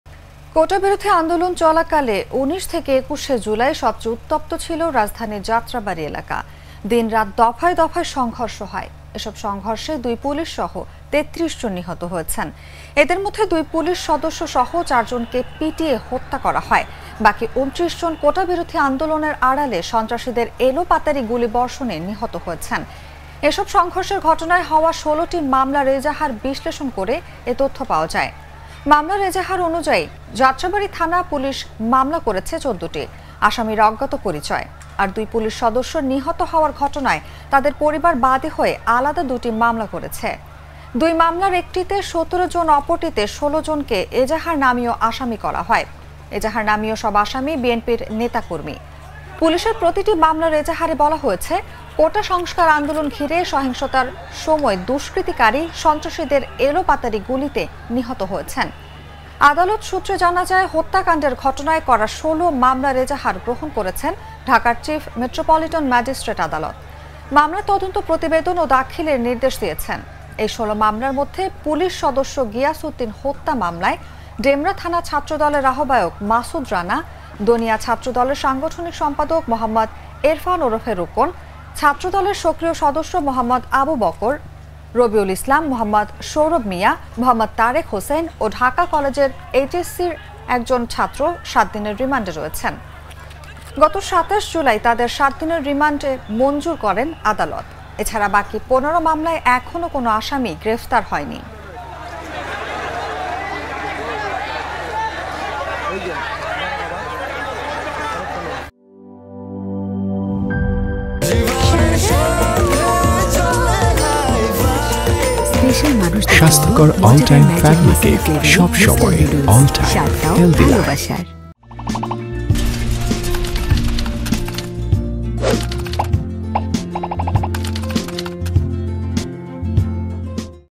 যাত্রাবাড়ীতে অধিকাংশই গু*লিতে নি*হ*ত | Quota Protest | Students | Jatrabari || Jago News
কোটাবিরোধী আন্দোলন চলাকালে ১৯-২১ জুলাই সবচেয়ে উত্তপ্ত ছিল রাজধানীর যাত্রাবাড়ী এলাকা। দিন-রাত দফায় দফায় সং-ঘ-র্ষ হয়। এসব সংঘর্ষে দুই পুলিশসহ ৩৩ জন নি-হ-ত হয়েছেন।